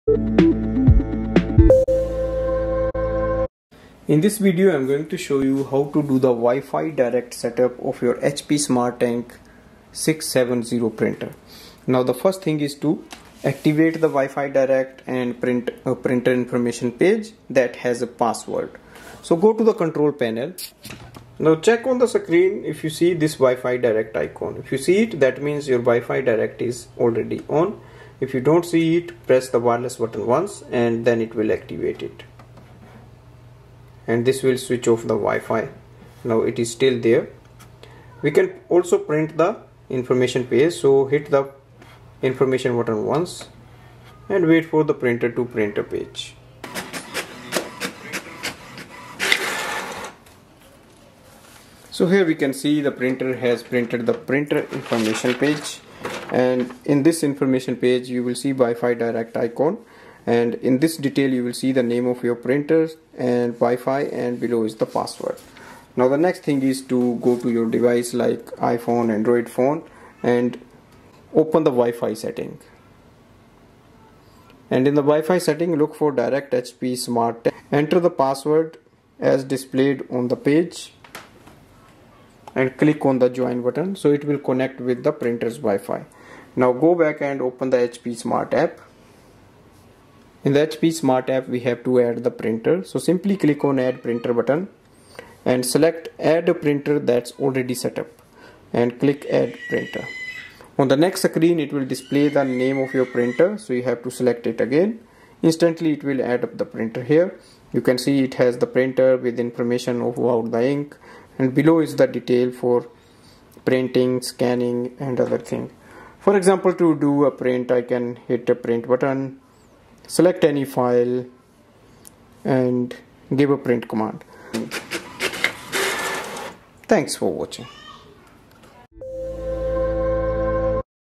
HP Smart Tank 670 WiFi Direct Setup.
This video shows how to do the wifi direct setup of your hp smart tank 670 all in one printer.